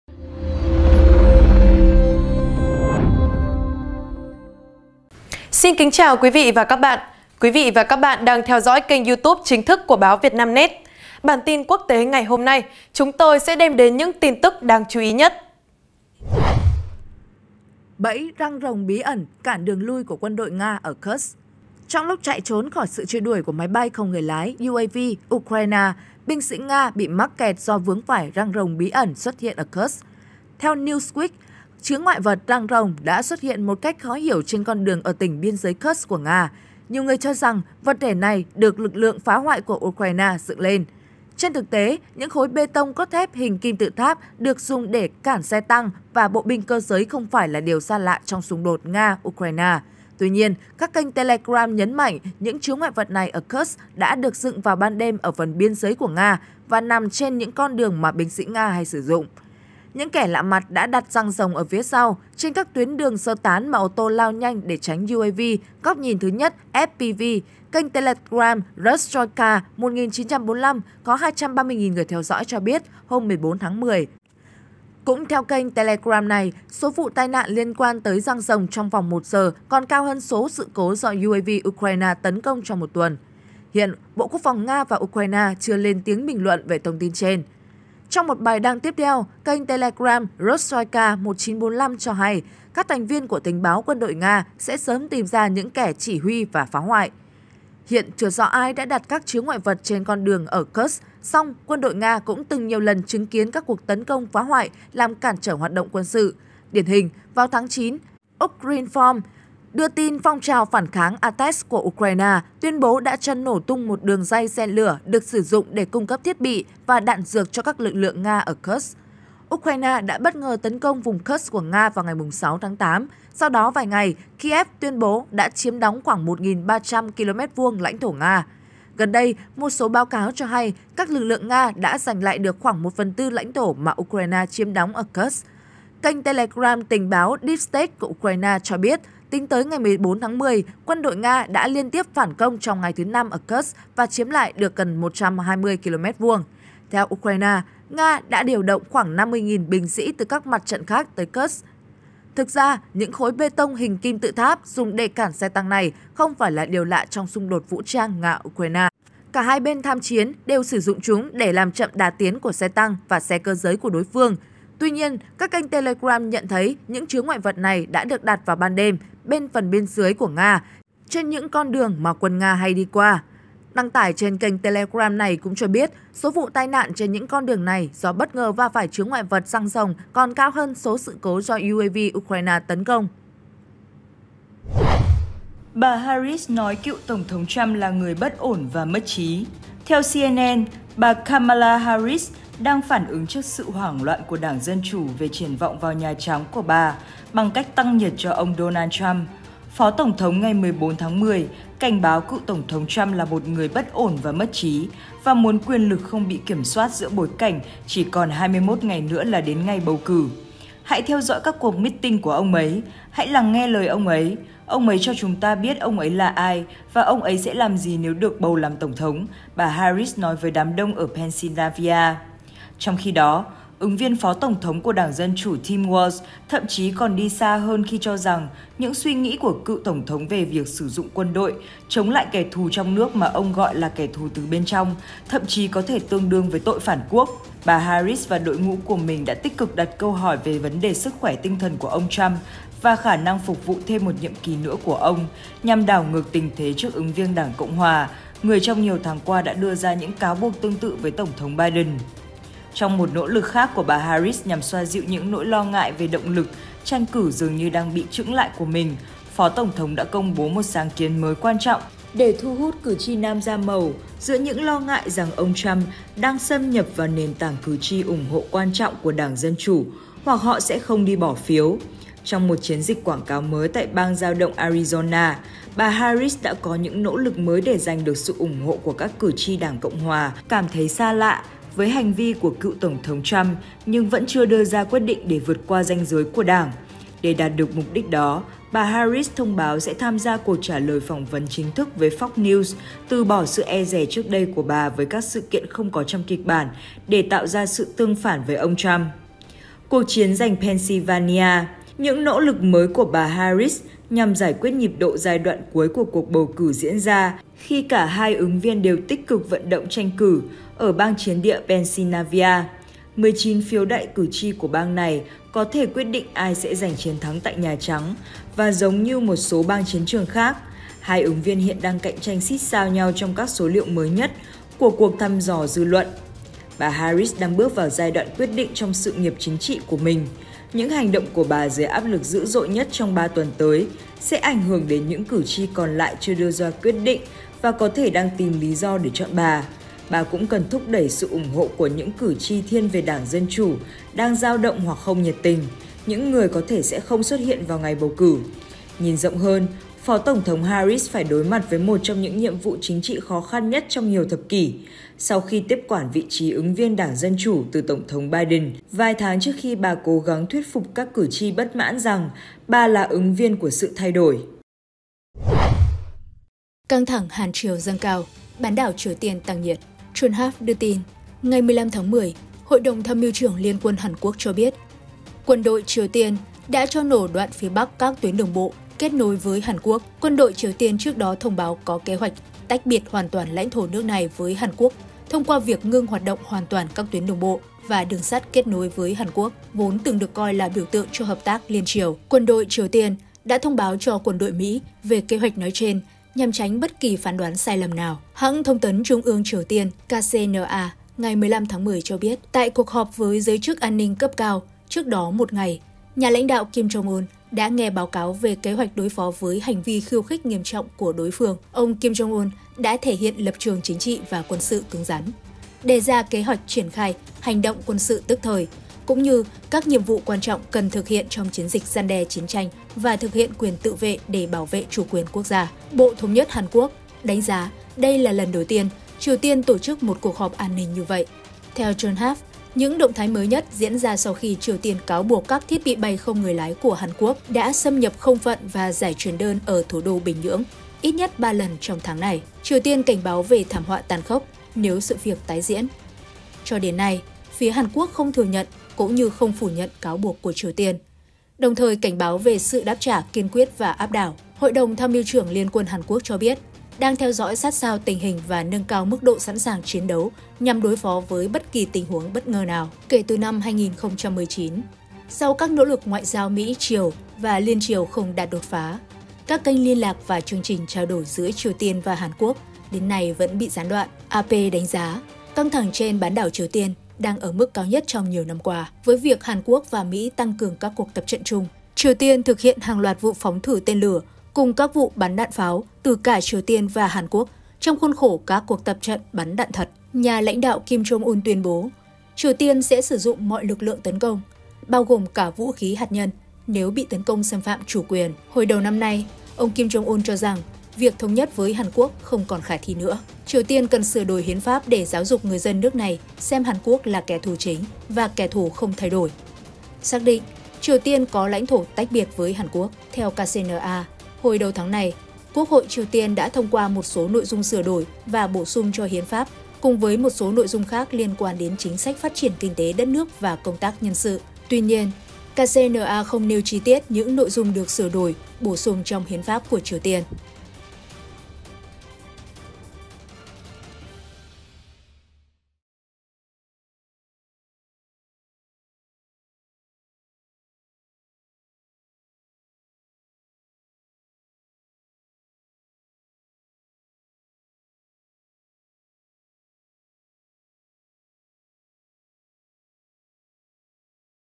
Bí ẩn bẫy ‘răng rồng’ cản đường lui của quân đội Nga ở Kursk | Báo VietNamNet
Theo Newsweek, chướng ngại vật “răng rồng” đã xuất hiện một cách khó hiểu trên con đường ở tỉnh biên giới Kursk của Nga. Nhiều người cho rằng, vật thể này được lực lượng phá hoại của Ukraine dựng lên.

Trên thực tế, những khối bê tông cốt thép hình kim tự tháp được dùng để cản xe tăng và bộ binh cơ giới không phải là điều xa lạ trong xung đột Nga - Ukraine. Tuy nhiên, các kênh Telegram nhấn mạnh những chướng ngại vật này ở Kursk đã được dựng vào ban đêm ở phần biên giới của Nga, và nằm trên những con đường mà binh sĩ Nga hay sử dụng.